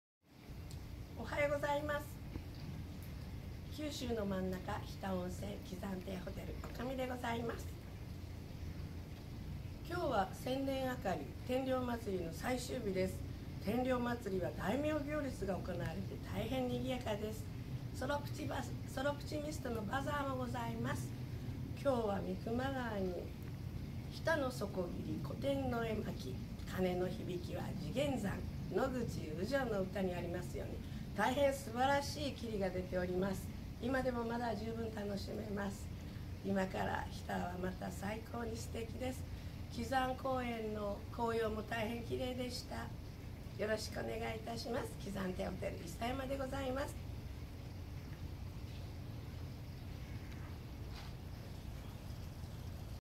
日田天領まつり、千年あかり最終日 大名行列 国際ソロプチミスト日田のバザー
本日、日田天領まつり、千年あかりの最終日です。
大名行列や国際ソロプチミスト日田のバザーもございます。